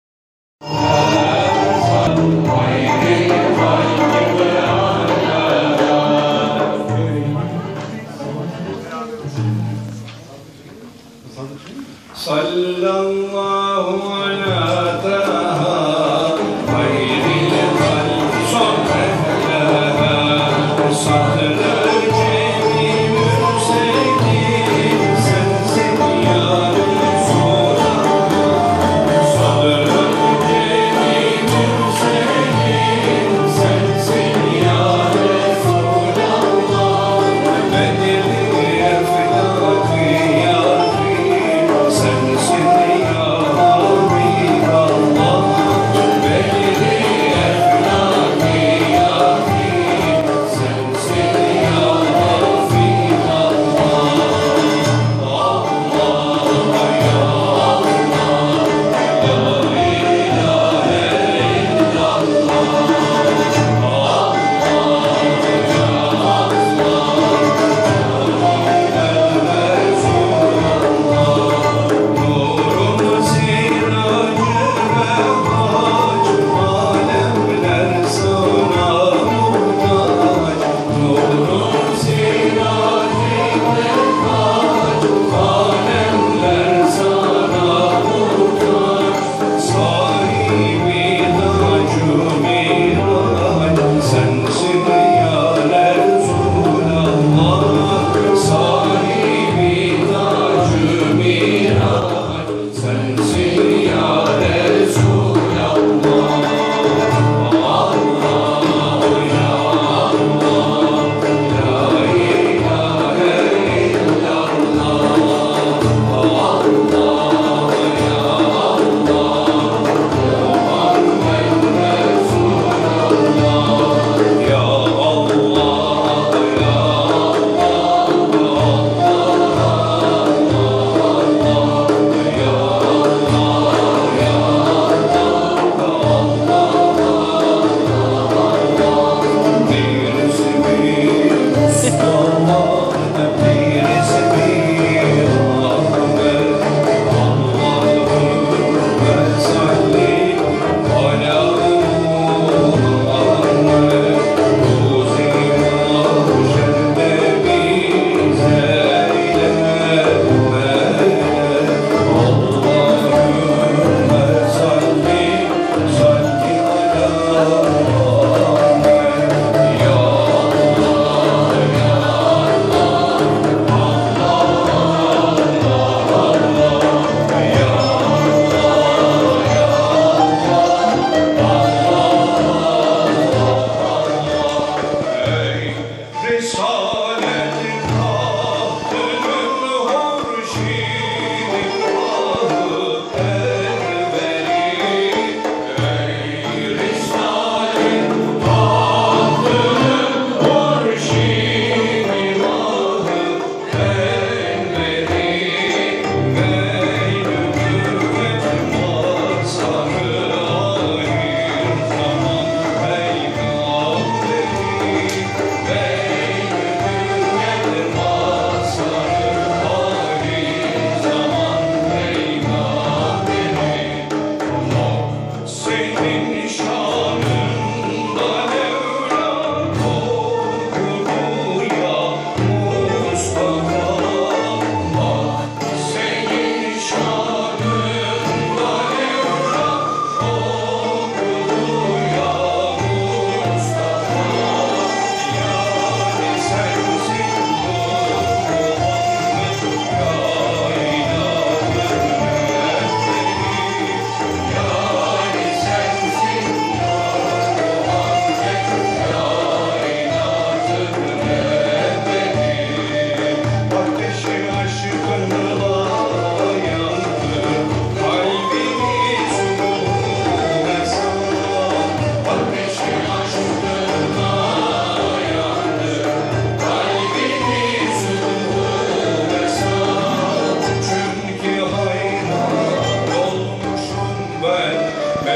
Ahmet Özhan / Bahçeşehir Üniversitesi / İlahi Konseri / Prova/Süleyman KURUÇAY
Bahçeşehir Üniversitesi Türk Müziği Topluluğu Konseri -Konuk Sanatçı Ahmet Özhan